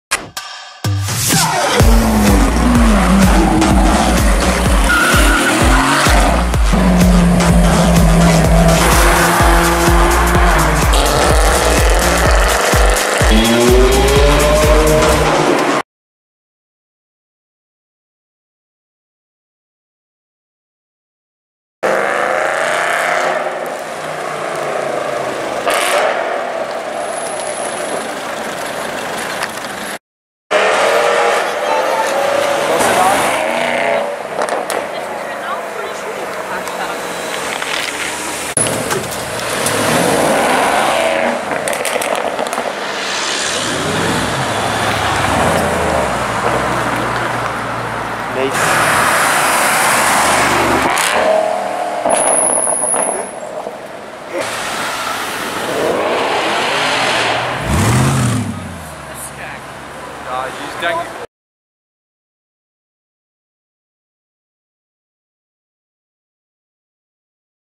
EXTREMELY LOUD Porsche Panamera GTS in Düsseldorf!
I'M EATING MY CAMERA IF THIS DOESN'T HAVE A AFTERMARKET EXHAUST. This car was so loud, and it scared the shit out of us with that downshift at 0:49. In this video you can hear it accelerate and downshift a lot! There was also a Mercedes SL55 AMG following it in the Königsallee.

Specs:
Top Speed: 288 km/h
Acceleration: 4.5 sec.
BHP: 430 hp
Torque: 520 Nm @ 6700 rpm
Weight: 1920 kg
Engine: 4.8L V8, N/A

Thanx for watching my vid's!